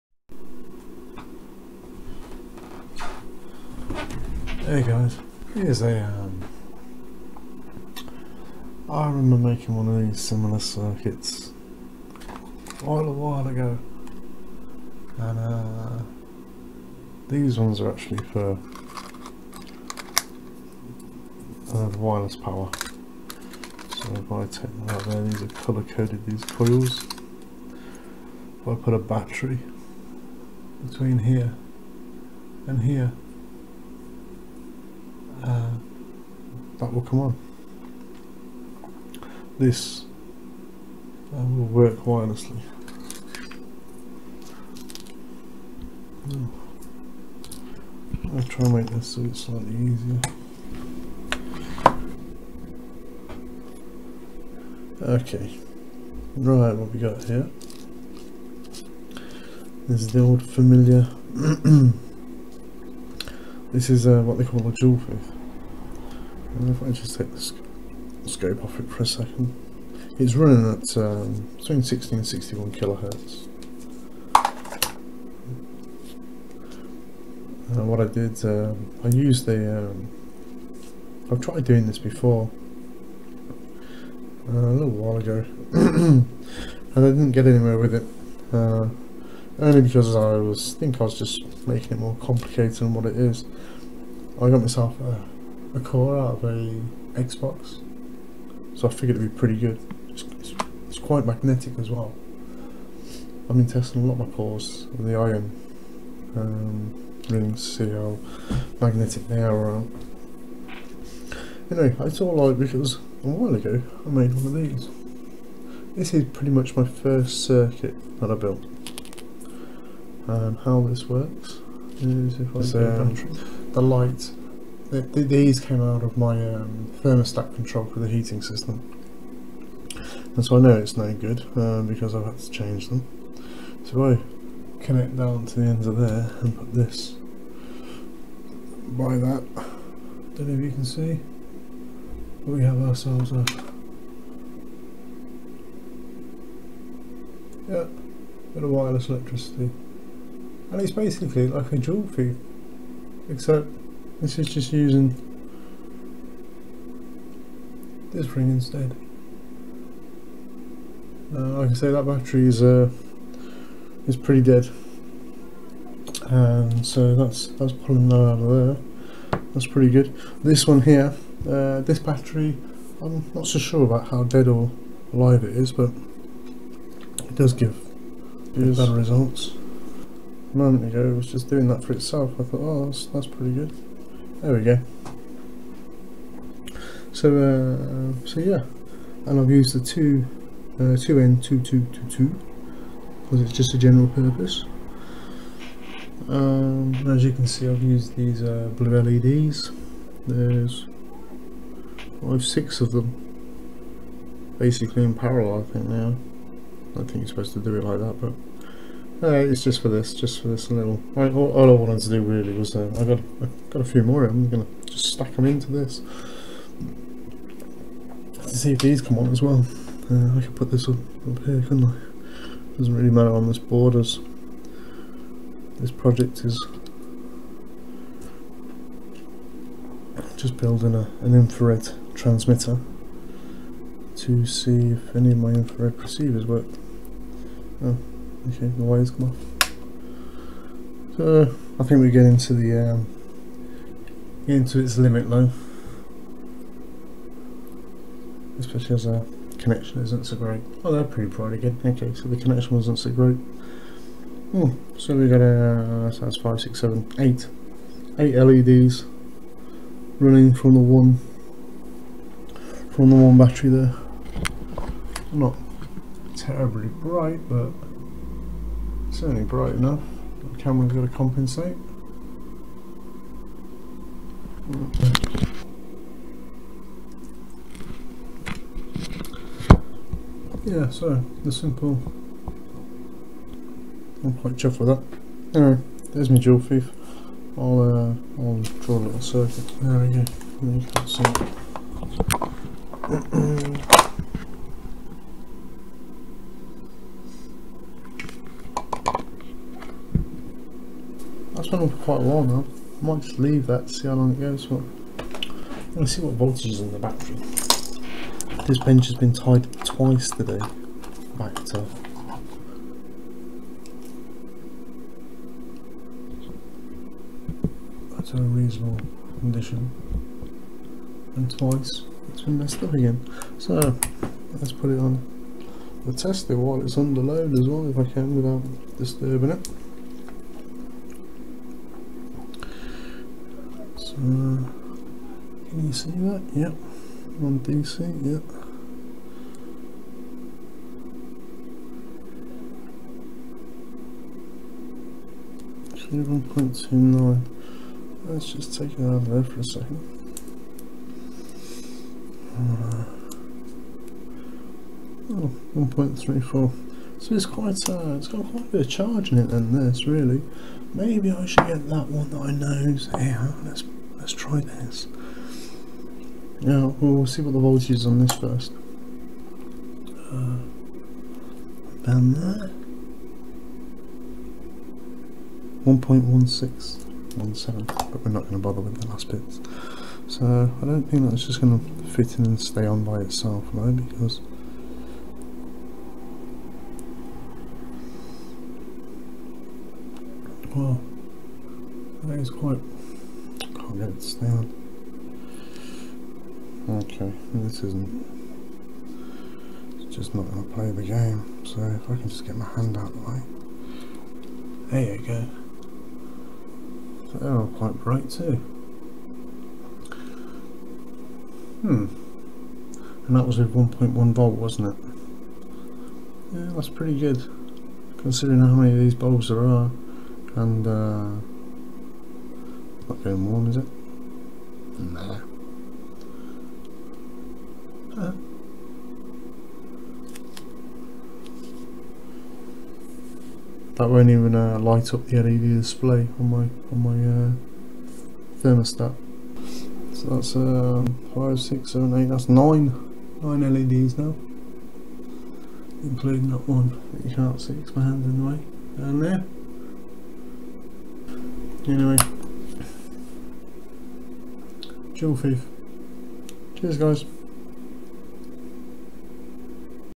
Joule Thief
8 months ago I tried to build a Joule Thief without success. I did manage wireless transfer with a similar circuit shown in this video.
Joule Thief circuit is copied from internet.